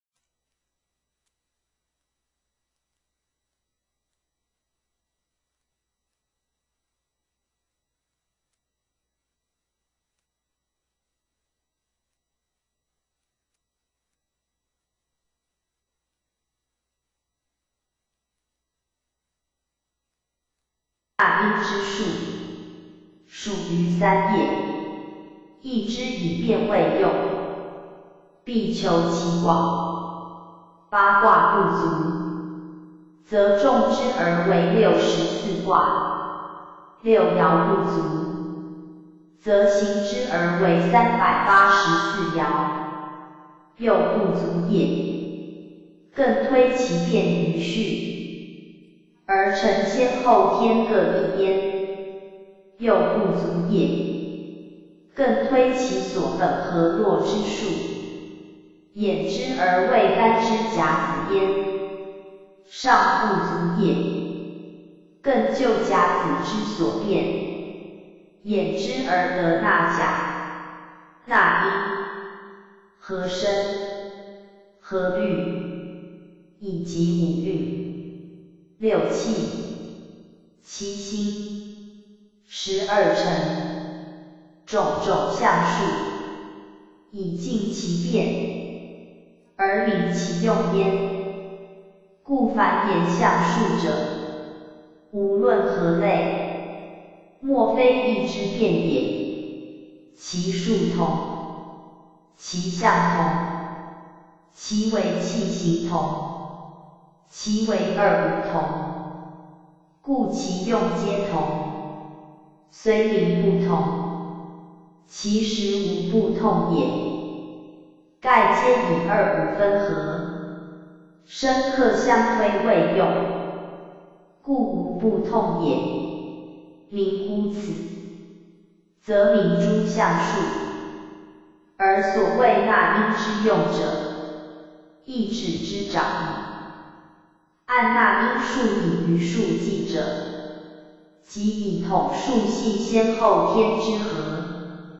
易經證釋上經第2冊圖象、伏羲文王八卦、各易大旨_摘錄--蓋易者以變易為主旨。凡天下之變有三。即一生二。二生三。三生萬物是也。一屬先天。二屬後天。三則先後天之合。納 音之數。屬于三也。易之以變為用。必
...必求其廣。八卦不足。則重之而為六十四卦。六爻不足。則行之而為三百八十四爻。又不足也。更推其變于序。而成先後天各易 焉。又不足也。更推其所本河洛之數。衍之而為干支甲子焉。尚不足也。更就甲子之所變。衍之而得納甲。納音。合聲。合律。以及五運。六氣。七星。十二辰。種 種象數。以盡其變。而明其用焉